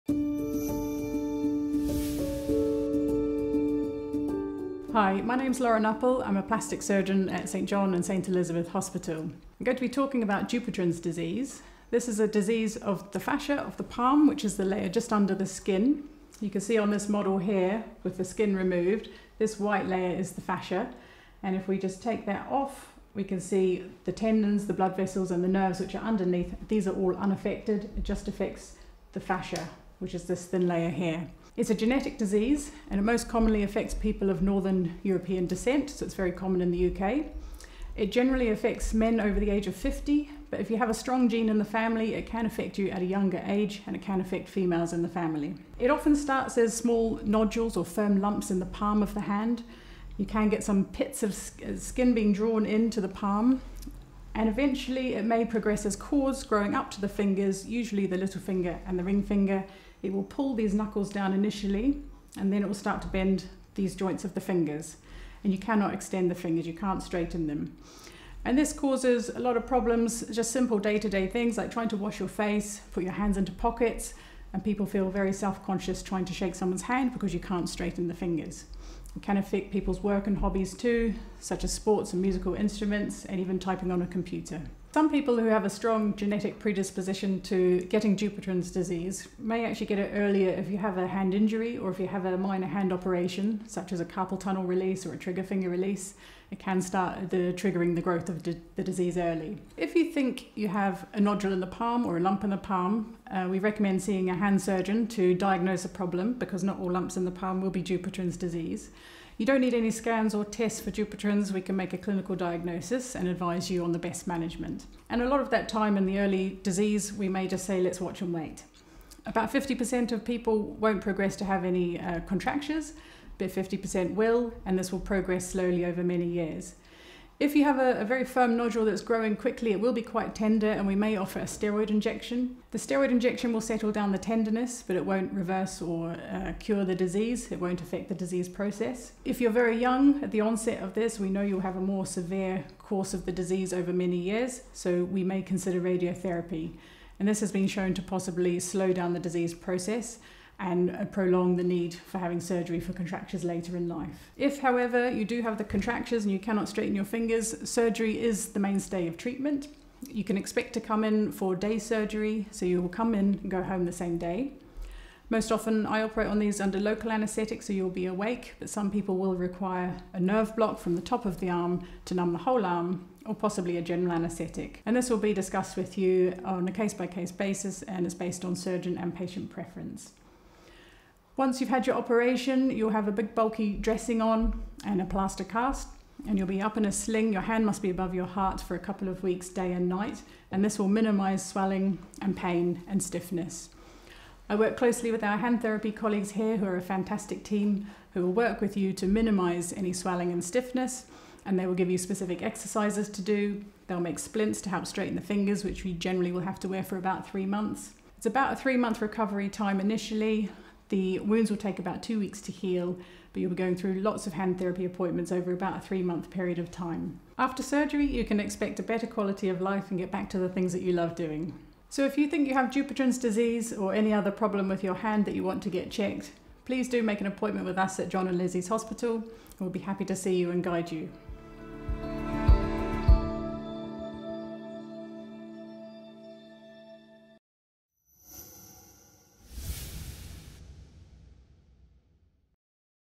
Dupuytren's Disease explained
Join Miss Lauren Uppal, Plastic and Reconstructive Surgeon, as she delves into the details of Dupuytren’s disease and its treatment methods here at St John & St Elizabeth Hospital.

If you think you might have Dupuytren’s disease, get in touch with our Hand & Wrist Clinic on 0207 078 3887